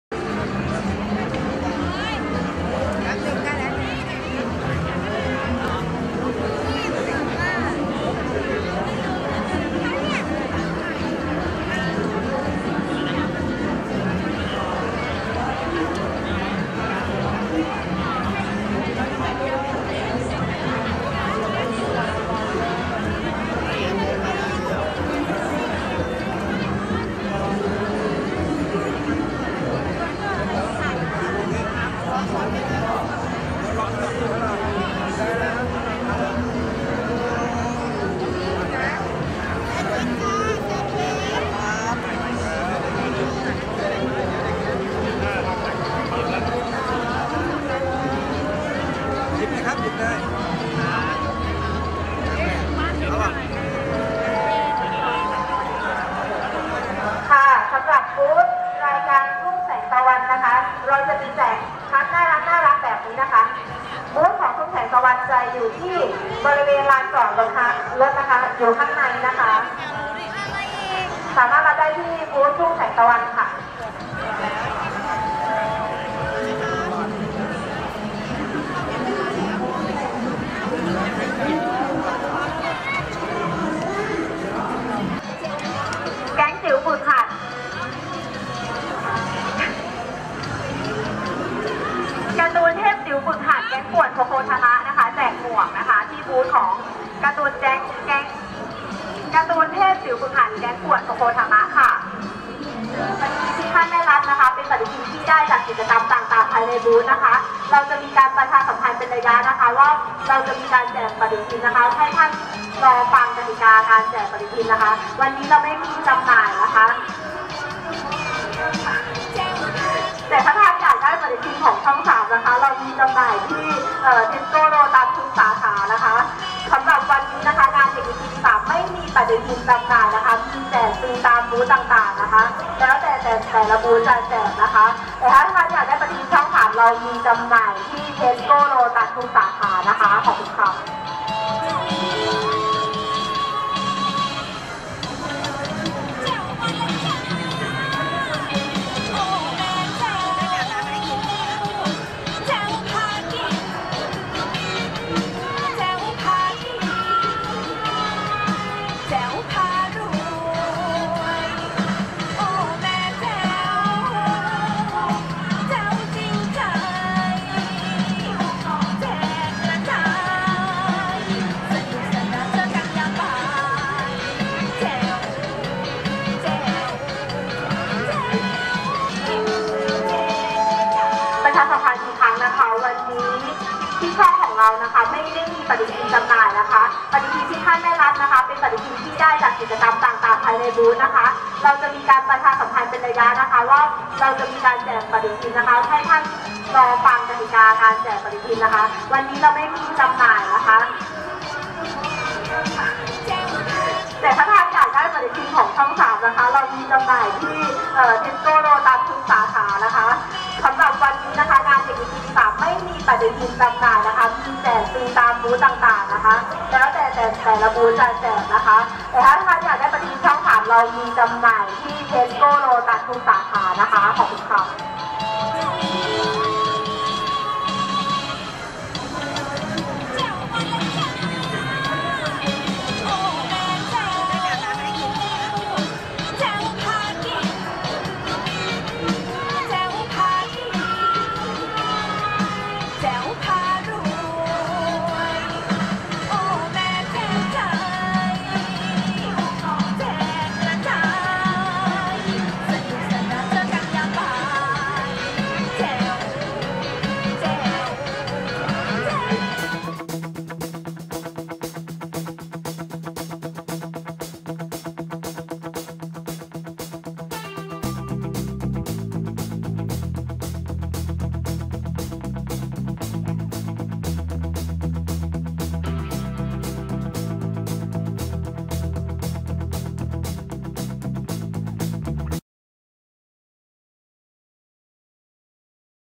เด็กดีทีวีสีช่อง3 สถานีวิทยุโทรทัศน์ ไทยทีวีสีช่อง3 อาคารมาลีนนท์ ถ.พราม4
สถานีวิทยุโทรทัศน์ไทยทีวีสีช่อง3
จัดเต็มเพื่อคุณหนูๆเด็กดีทีวีสีช่อง3ปี2561 ภายในงานเเจกอาหารฟรี ซุ้มสมาคมเดอะเชฟ
ณ อาคารมาลีนนท์ ทาวเวอร์ ถนนพระราม4 แขวงคลองตัน เขตคลองเตย กรุงเทพมหานคร